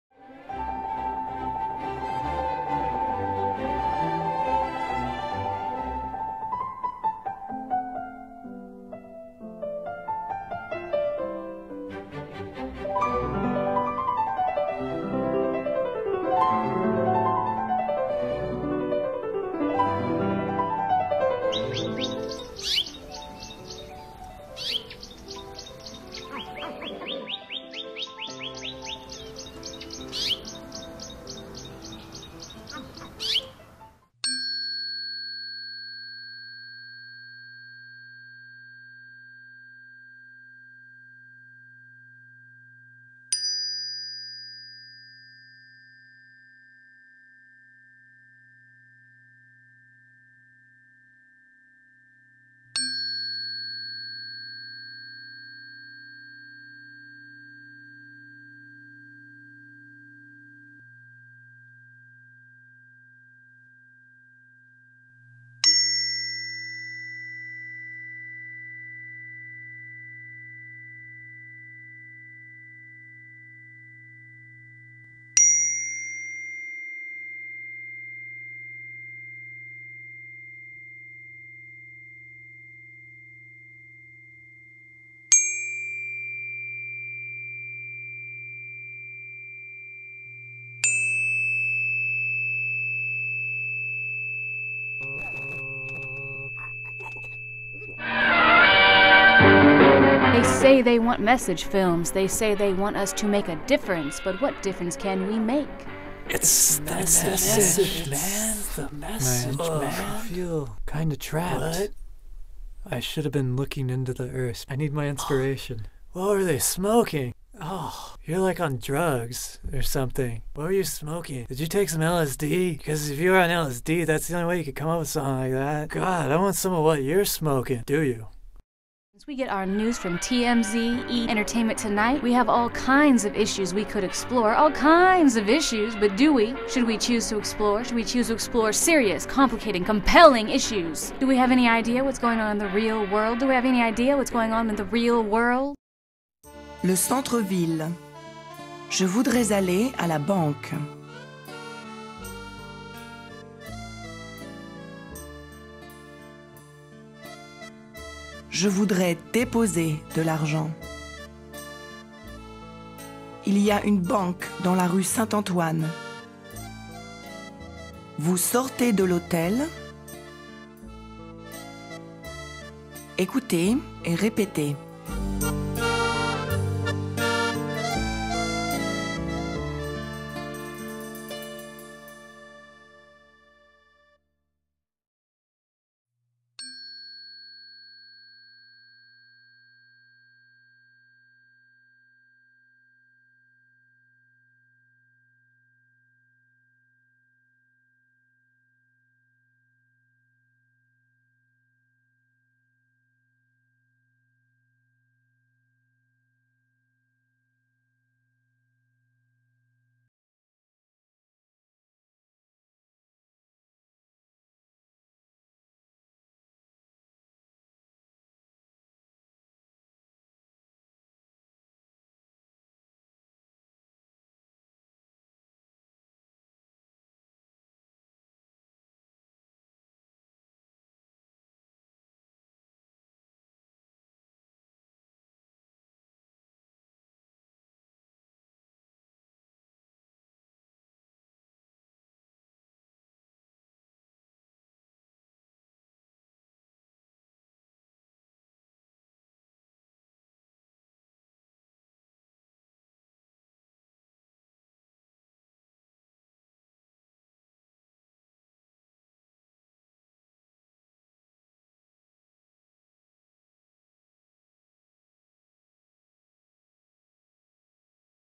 A Day With Mr. X
An experimental 16mm film exploring human poses and juxtaposition of image and sound. Limitation was that it had to be hand held.